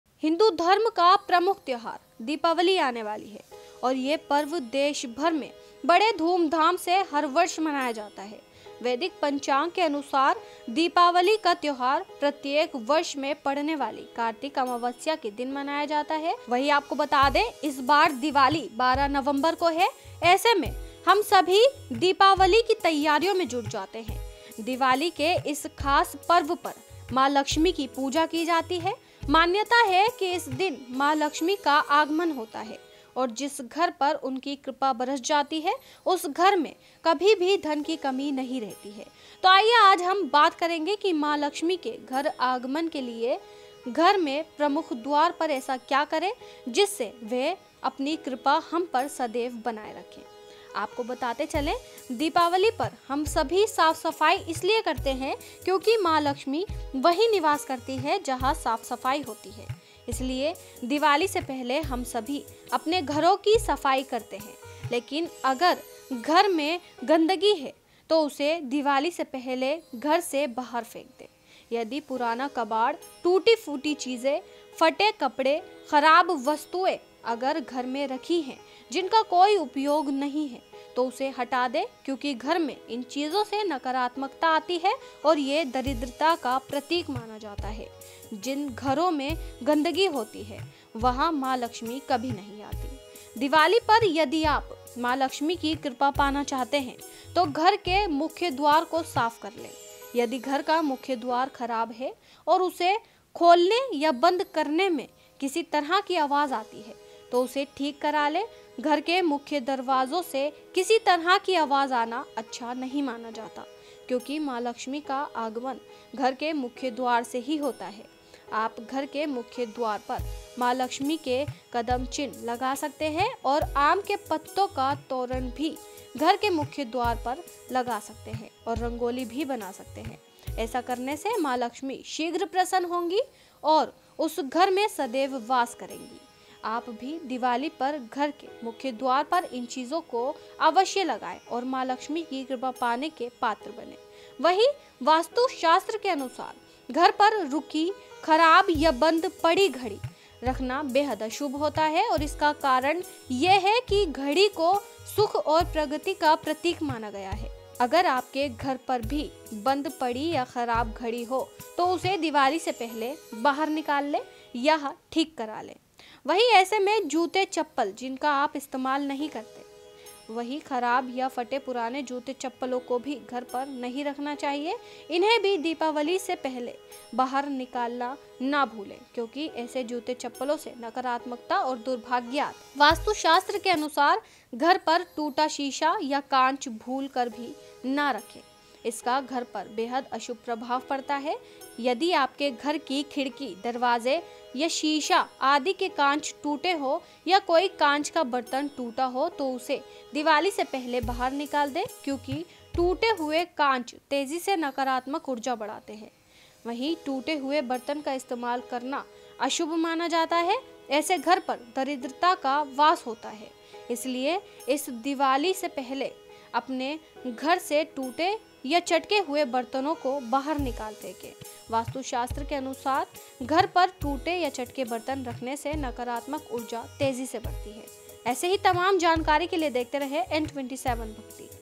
दिवाली से पहले घर से निकाल फेंके ये कबाड़
ऐसे घर पर दरिद्रता का वास होने लगता है. इसलिए इस दिवाली से पहले अपने घर से टूटे या चटके हुए बर्तनों को बाहर निकाल फेंके. वास्तु शास्त्र के अनुसार, घर पर टूटे या चटके बर्तन रखने से नकारात्मक ऊर्जा तेजी से बढ़ती है.